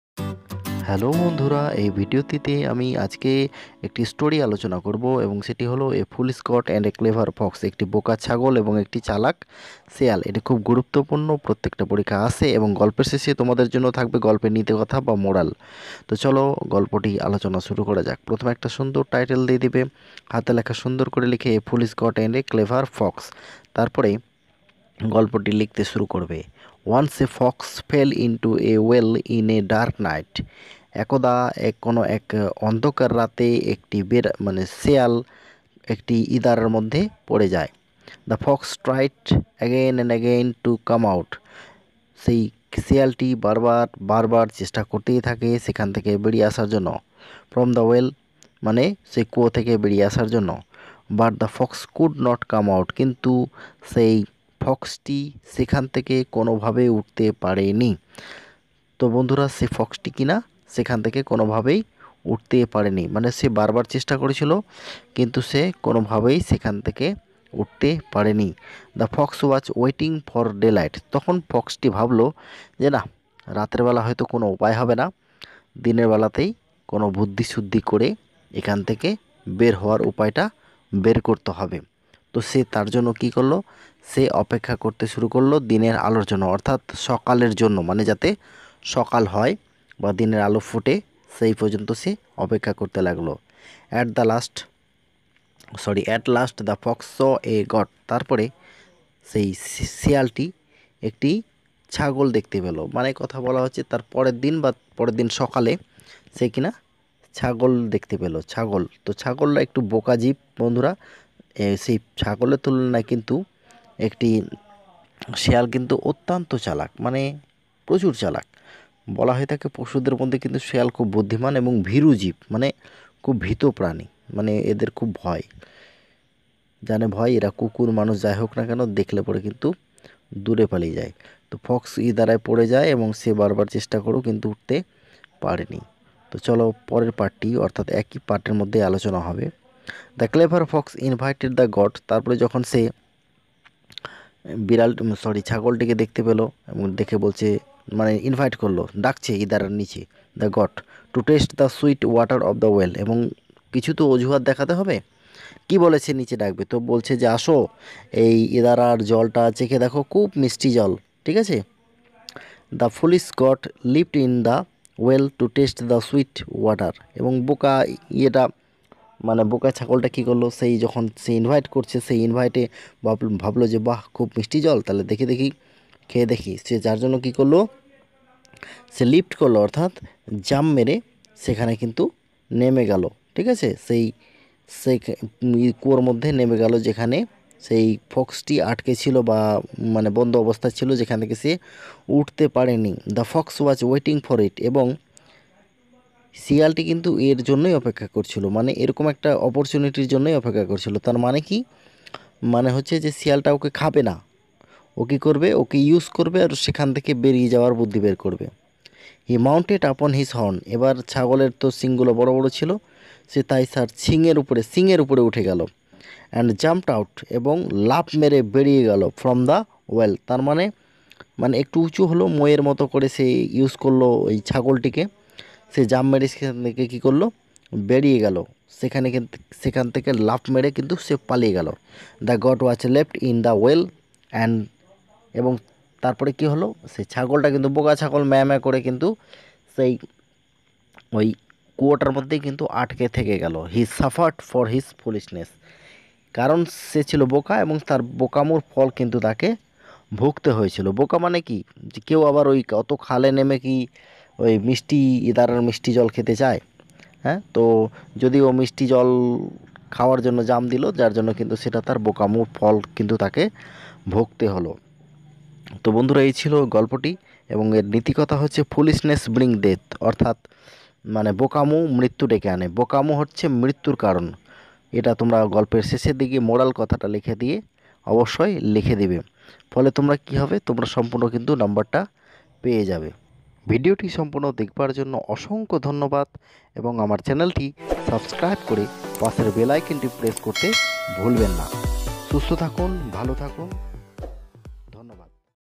Foolish Goat and Clever Fox story writing in English
Clever Fox and foolish Goat story writing